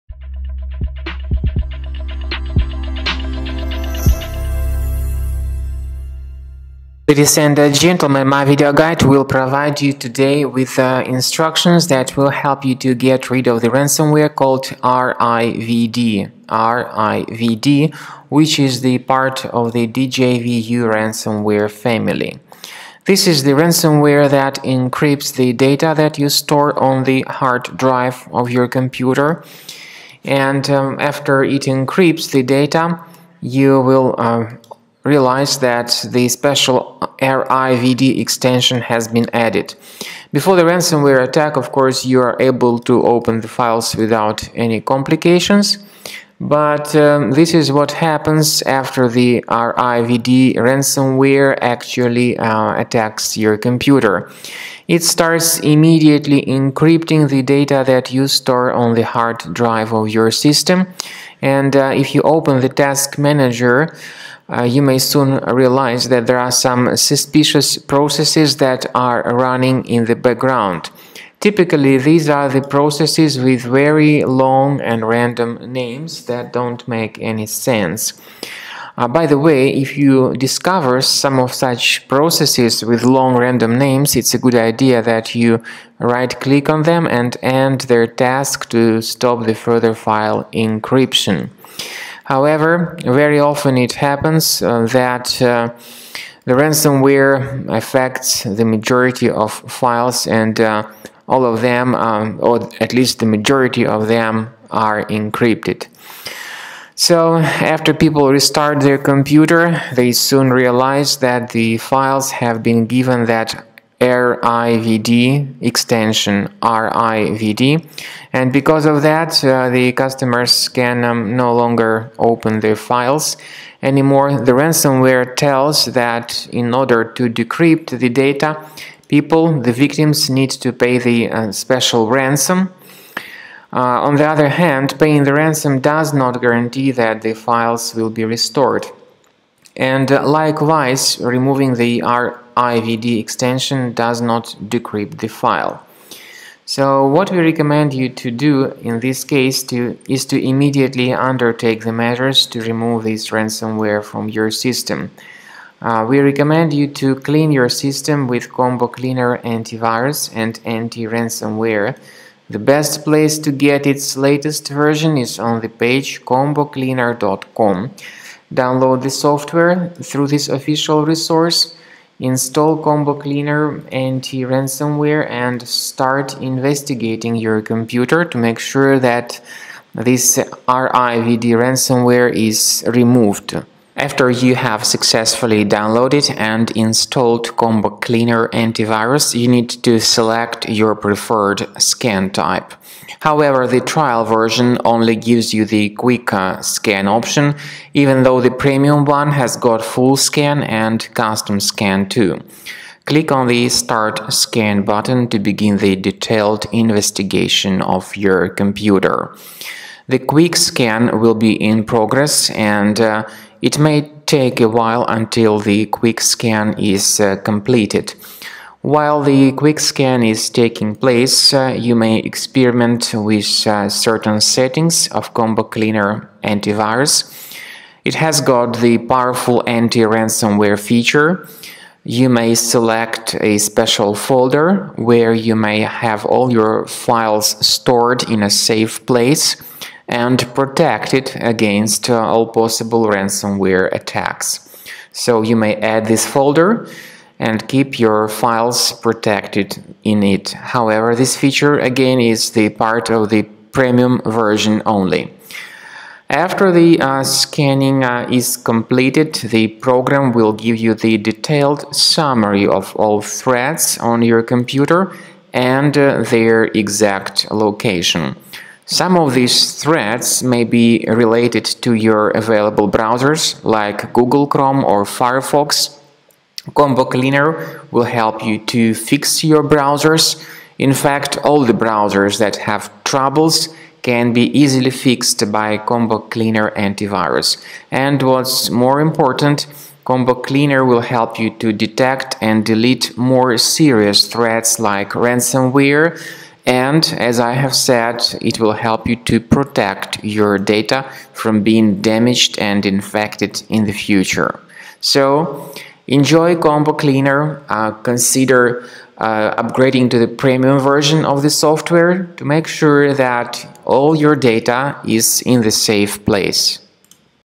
Rivd ransomware removal. How to recover .rivd files?
Rivd is a ransomware-type threat that belongs to the DJVU ransomware family. It encrypts the data and then adds a special .rivd extension to the modified files. It then tells its victims to pay the ransom ($980) for file restoration. However, paying the ransom does not guarantee file restoration. We recommend you to remove it with Combo Cleaner Antivirus and get your data protected with its help in the future.

Rivd stands for the name of the ransomware sample that is member of the Djvu ransomware family. Rivd encrypts data and appends the ".rivd" extension to filenames (for instance, it renames "3.jpg" to "3.jpg.rivd", "4.jpg" to "4.jpg.rivd"). Also, it generates the "_readme.txt" file containing contact and payment data. However, paying the ransom does not guarantee file recovery.